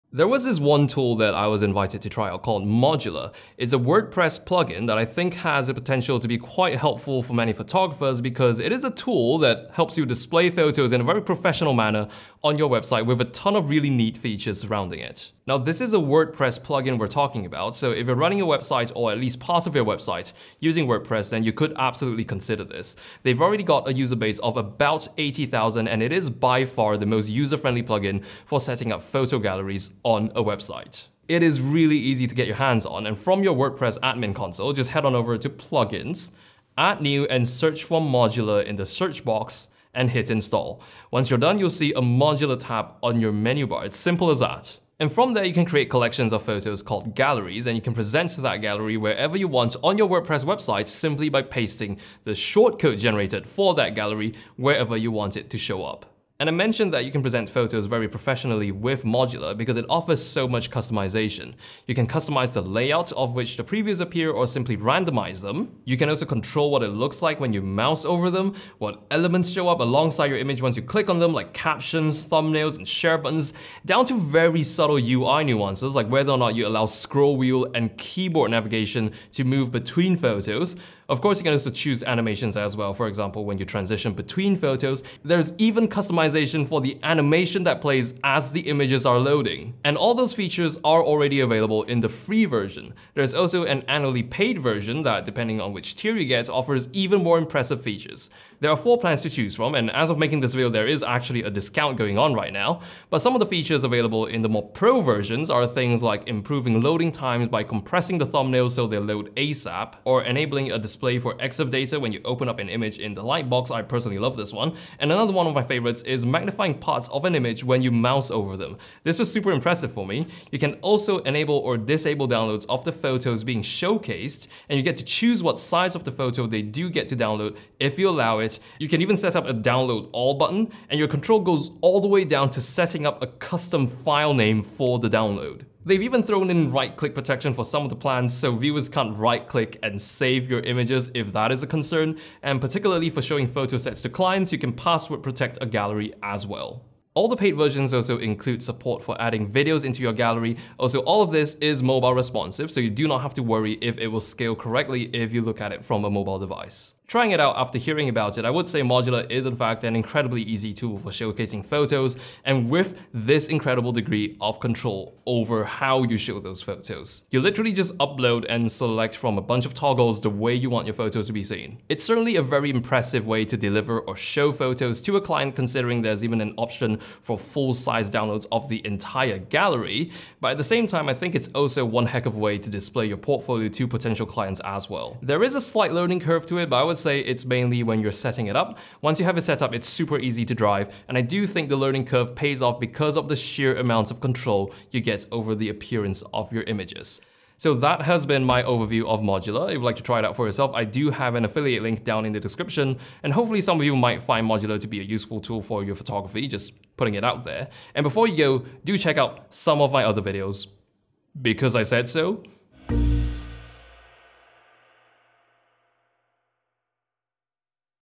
Best Tool for Showcasing Photos Online? Modula Overview (Wordpress Plugin)
Modula might just be the most detailed tool I've come across for displaying photos online. Definitely worth checking out for photographers.

Special Patron shoutout to TheResistorNetwork for supporting the channel!